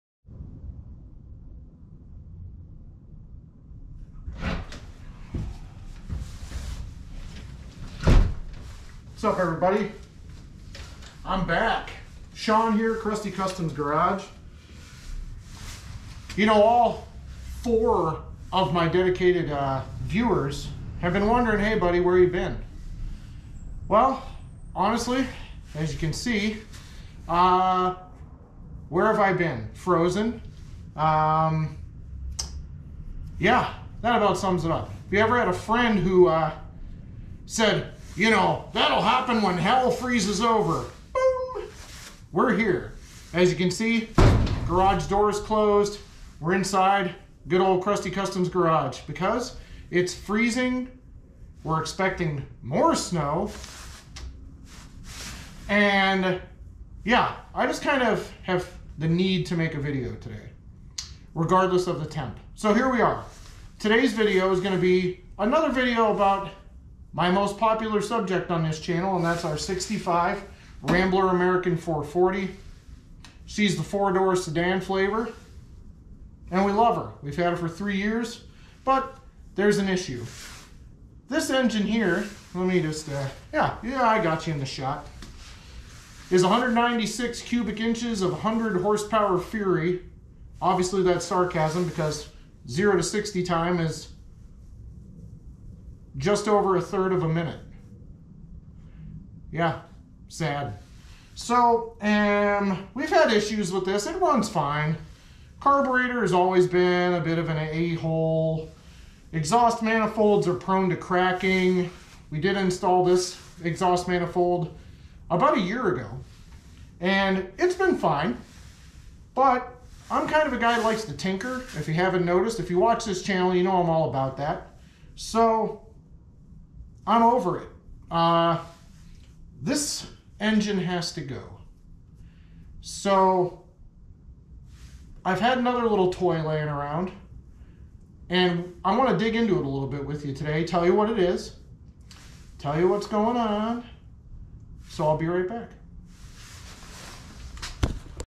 Part 1- Our 1965 Rambler American 440 Sedan's heart transplant gets its first examination
Lets get moving on this Rambler. I need a more reliable car and this new heart should do it. Thanks.